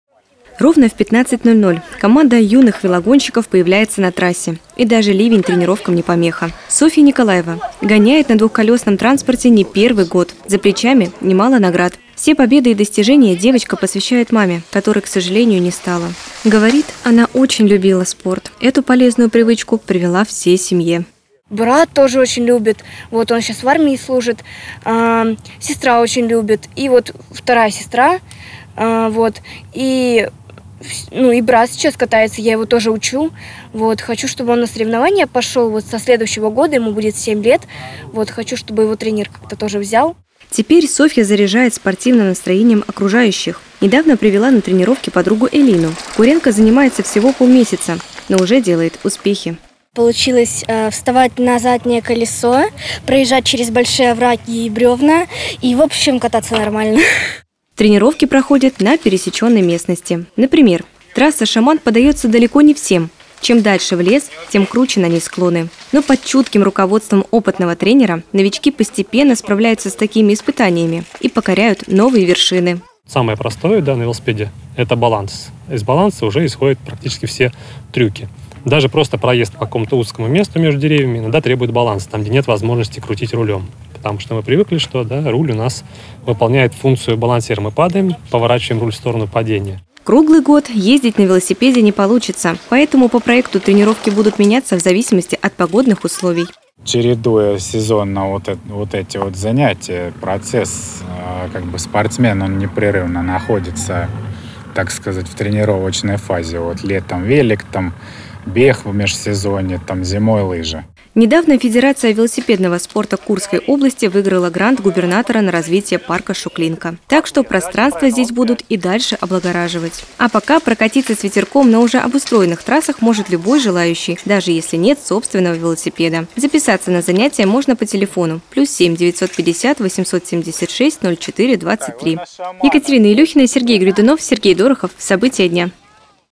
В Курске для детей проводят бесплатные тренировки по велоспорту
Проводят летние каникулы с пользой. Каждый понедельник, среду и пятницу юные куряне приезжают в Шуклинку на бесплатные велотренировки. Кто-то крутит педали впервые, а кто-то уже давно увлекается ездой по пересечённой местности. Занятия проходят в рамках проекта «Доступная велосреда». Как на них записаться - узнала Екатерина Илюхина.